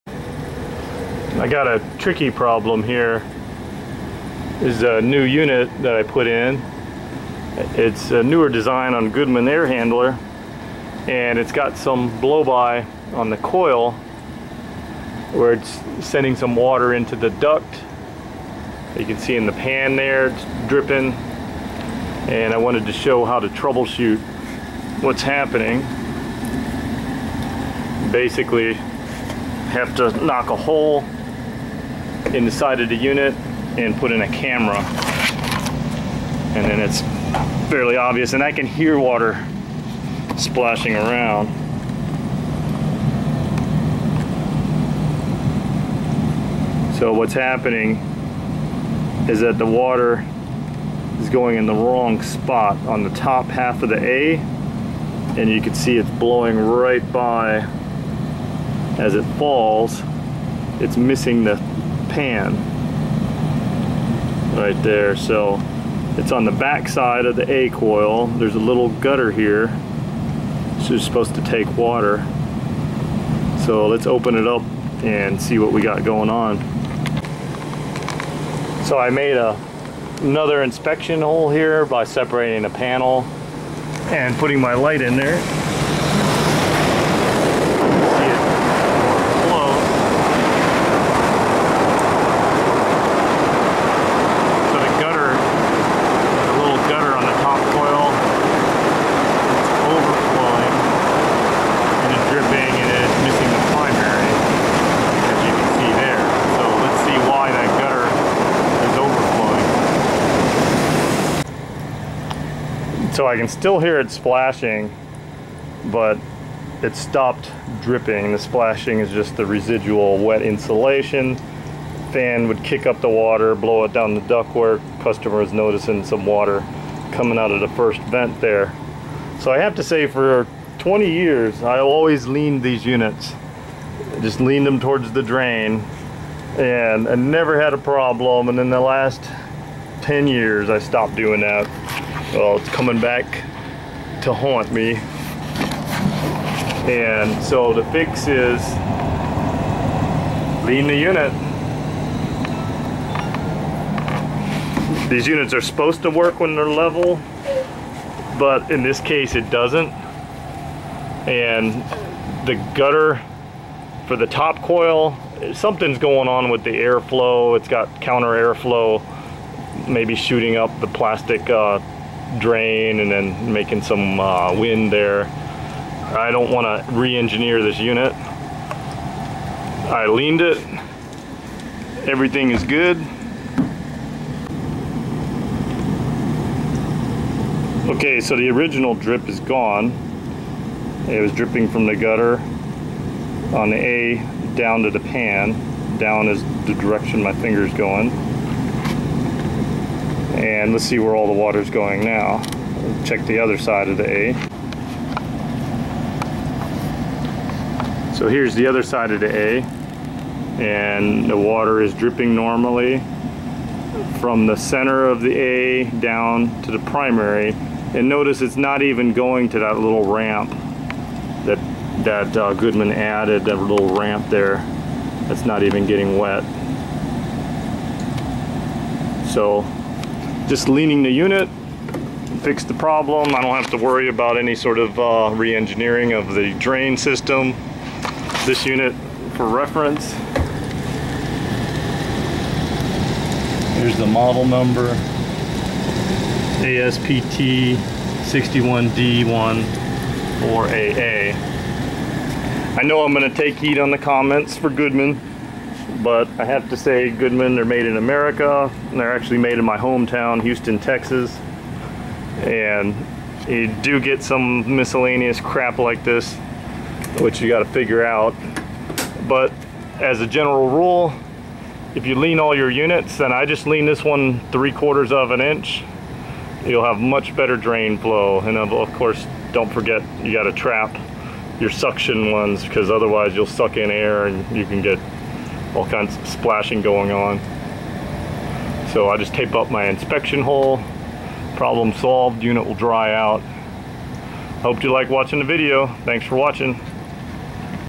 How to diagnose and fix water blow by leaks on Evaporators
Just lean the unit to the drain and you will have better drainage.  This is a Goodman ASPT air handler.